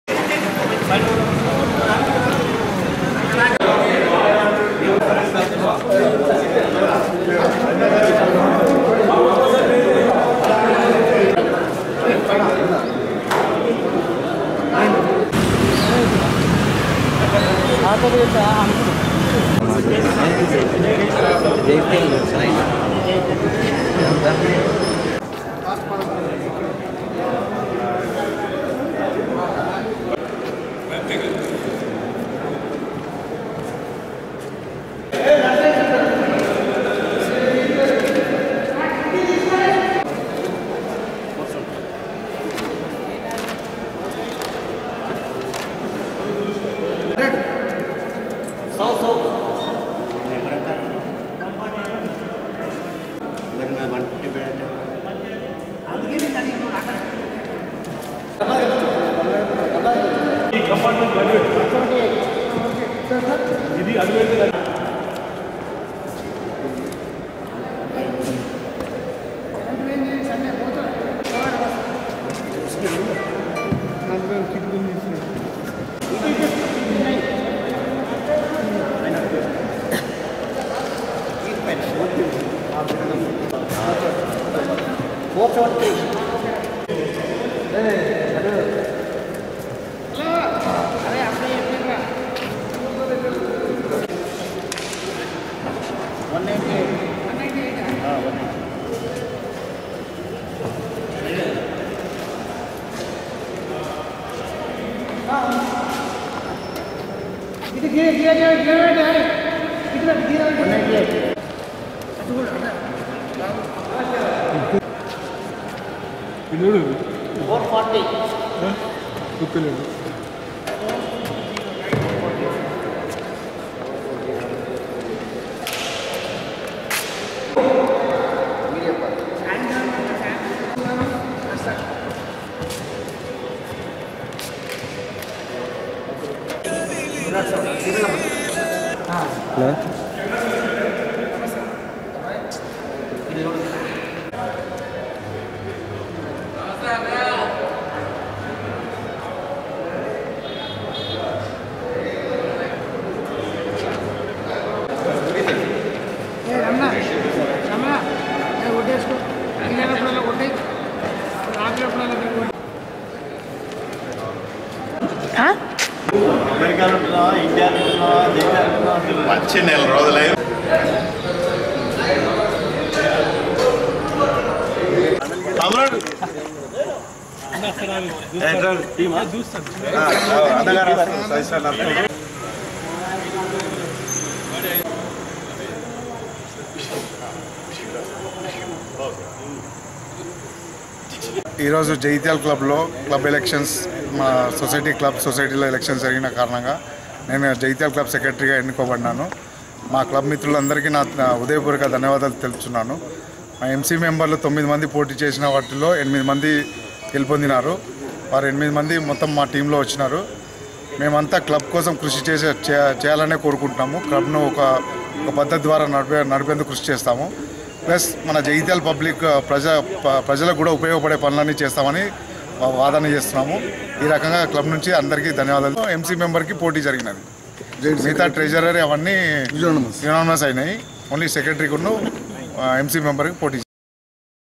19.1.2020| DSC | Jagtial |జగిత్యాల క్లబ్ ఎన్నికల్లో కార్యదర్శిగా గెలుపొందిన డాక్టర్ రామకృష్ణ రావు
| Durga Siti Cable | News |జగిత్యాల క్లబ్ ఎన్నికల్లో కార్యదర్శిగా గెలుపొందిన డాక్టర్ రామకృష్ణ రావు